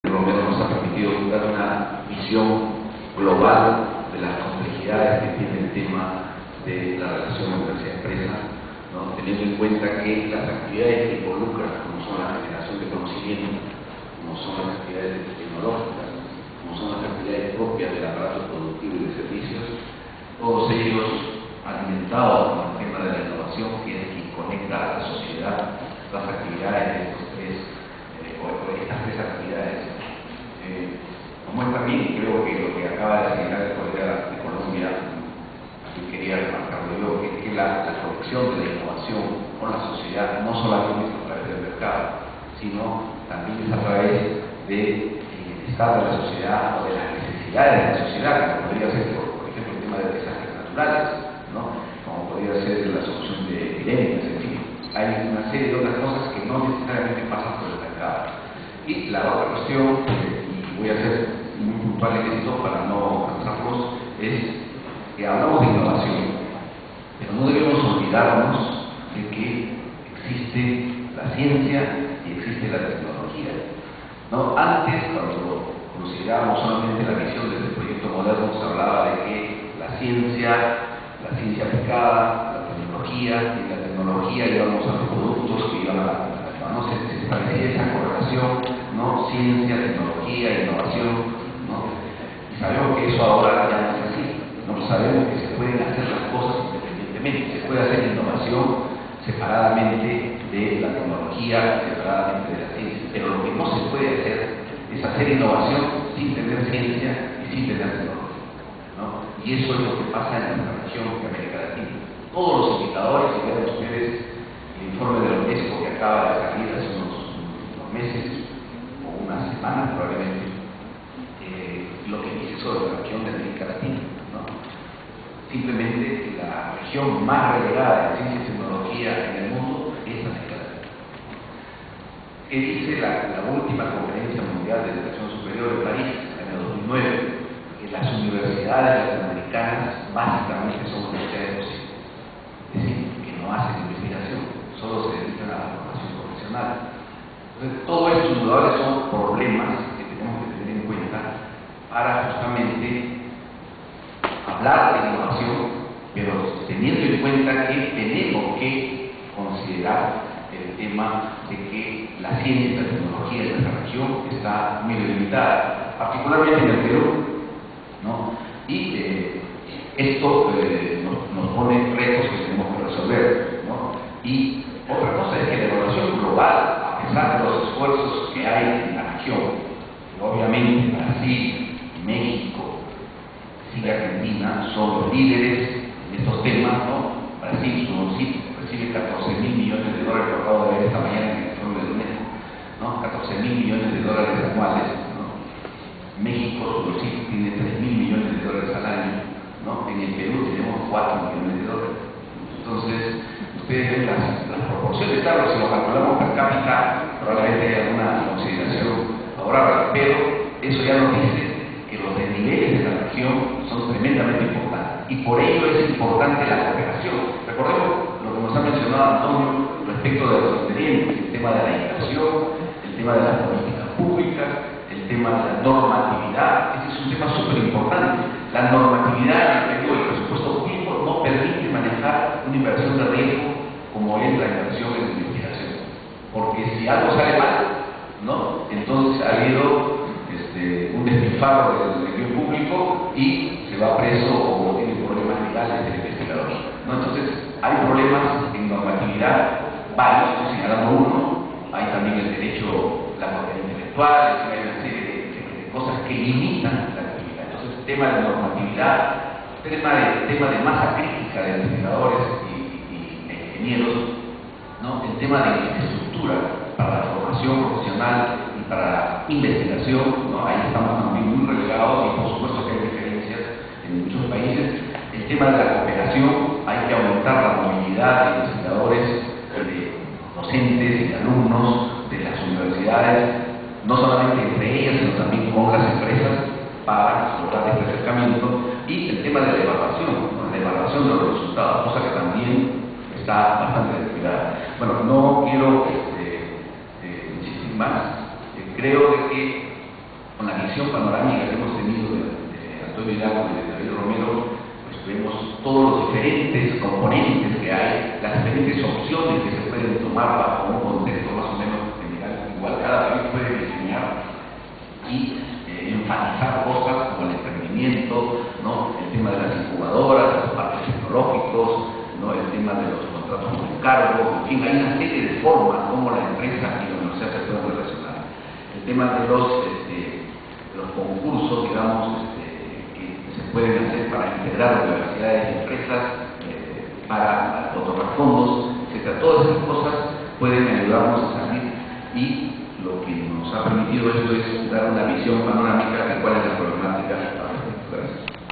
Comentarios de Jaime Avalos
Conferencia. "La relación universidad-empresa y la gestión de la innovación abierta".
Ponente: Dr. Antonio Hidalgo Nuchera. Universidad Politécnica de Madrid (UPM).
Organizador: Consejo Nacional de Ciencia, Tecnología e Innovación Tecnológica (CONCYTEC) - Perú.
Evento: III Congreso Nacional de Innovación.
Lugar: Cámara de Comercio de Lima. Perú.